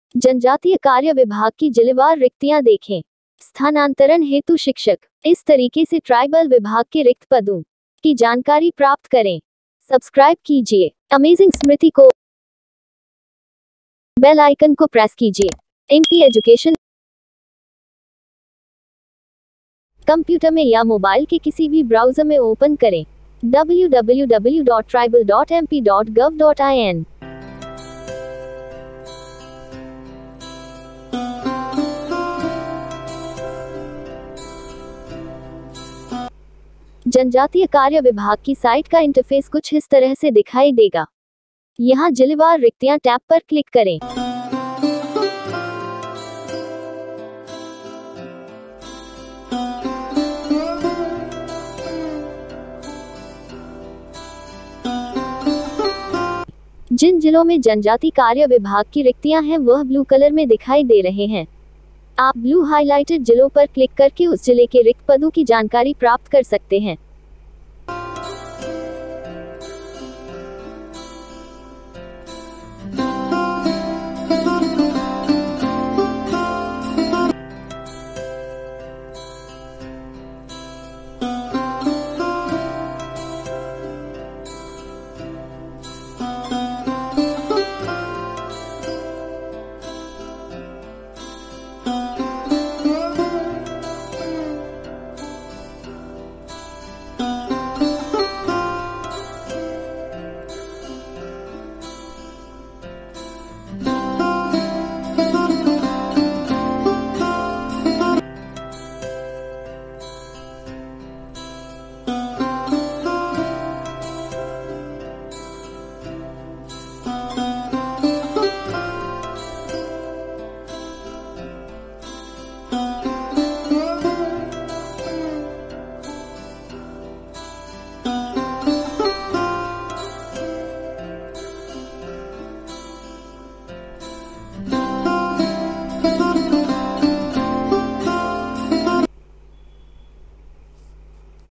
जनजातीय कार्य विभाग की जिलेवार रिक्तियां देखें/स्थानांतरण हेतु शिक्षक इस तरीके से देखें रिक्तयां
AmazingSmriti View district wise vacancies of Tribal Affairs Department / Teachers for transfer see vacancies in this way